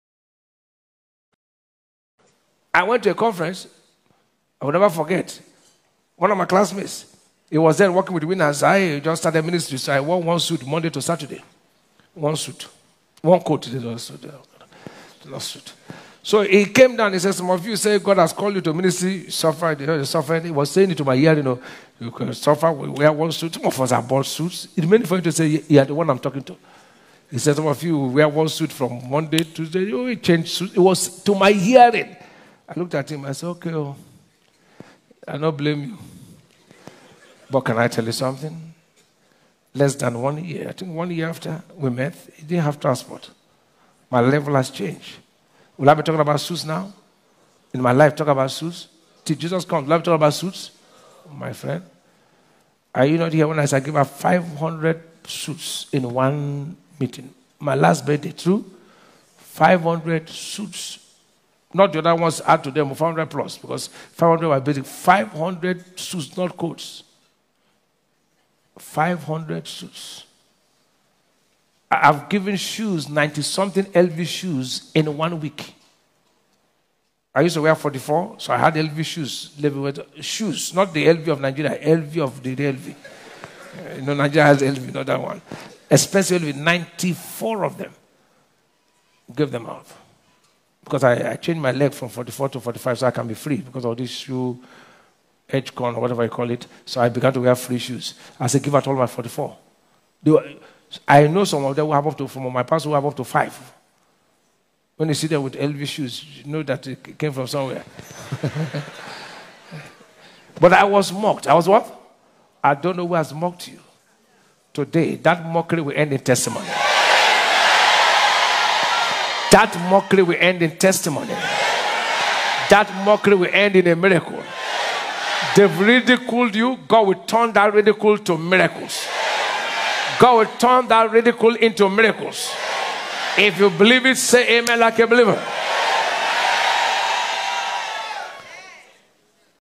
I GAVE OUT 500 SUITS FOR MY BIRTHDAY. I HAVE GIVEN OUT OVER 90 SHOES IN ONE WEEK - Pastor Ibiyeomie
I GAVE OUT FIVE HUNDRED SUITS IN MY LAST BIRTHDAY. I HAVE GIVEN OUT OVER NINETY LOUIS VUITTON SHOES IN ONE WEEK.
-Pastor David Ibiyeomie